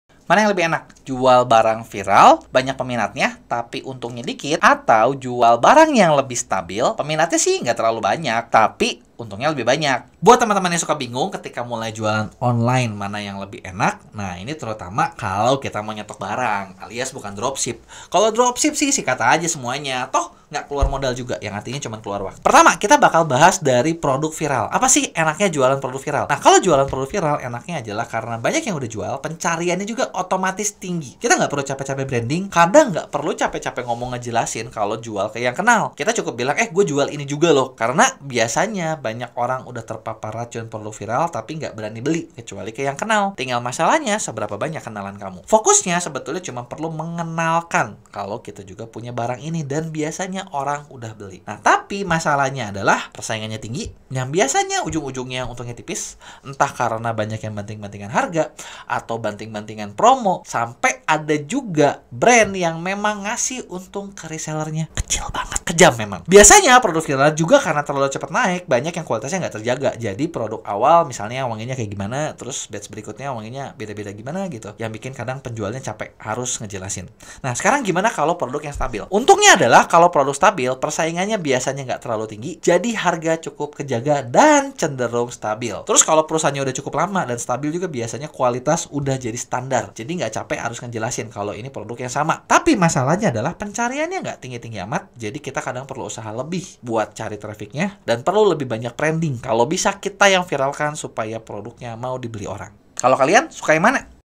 Mending jualan barang viral cepat laku / jual barang yang stabil tapi stok barang susah keluar
memang ada plus minusnya sih, tapi kalau dilihat lagi kayanya sama sama punya resiko untung dan rugi yang sama. menurut kamu?

Scale Up bisnis kamu dengan kerjasama marketplace bareng MEA